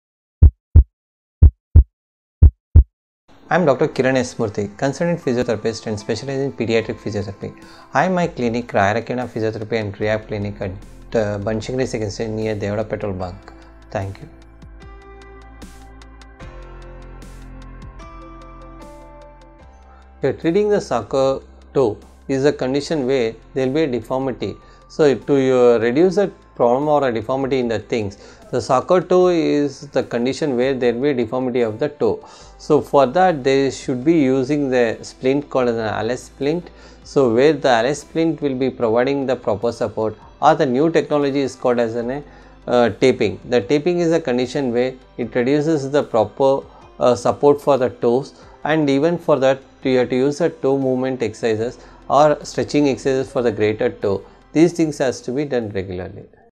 How to treat Soccer Toe? - Kiran Sundara Murthy
Treating soccer toe is a condition where there is a deformity in the toe. So for that they will be using a splint called as the Alex splint, which will be providing the correct support. The new technology is called as the taping. Taping is the condition where it reduces the proper support for the toes and for that you have to use the toe movement exercises or stretching exercises for the greater toe.